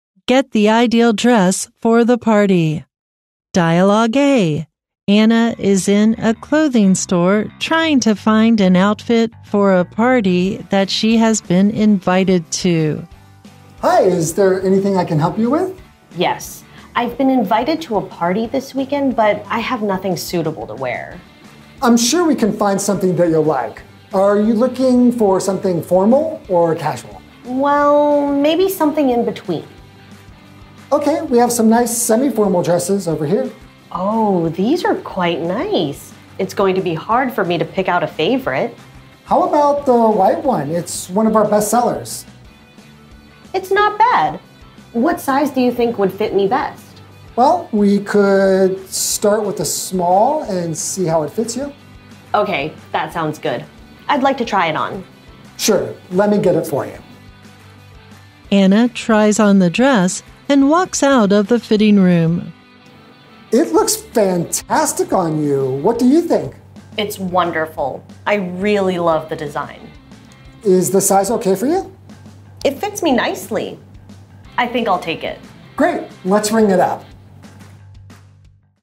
【買件漂亮洋裝去開趴】 2024.11.18 Get the Ideal Dress for the Party
C: Clerk A: Anna
Day 1
Anna is in a clothing store trying to find an outfit for a party that she has been invited to.
C: Hi, is there anything I can help you with?
A: Yes, I’ve been invited to a party this weekend, but I have nothing suitable to wear.
C: I’m sure we can find something that you’ll like. Are you looking for something formal or casual?
A: Well, maybe something in between.
C: OK, we have some nice semi-formal dresses over here.
A: Oh, these are quite nice. It’s going to be hard for me to pick out a favorite.
C: How about the white one? It’s one of our bestsellers.
A: It’s not bad. What size do you think would fit me best?
C: Well, we could start with a small and see how it fits you.
A: OK, that sounds good. I’d like to try it on.
C: Sure. Let me get it for you.
(Anna tries on the dress and walks out of the fitting room.)
C: It looks fantastic on you! What do you think?
A: It’s wonderful. I really love the design.
C: Is the size OK for you?
A: It fits me nicely. I think I’ll take it.
C: Great! Let’s ring it up.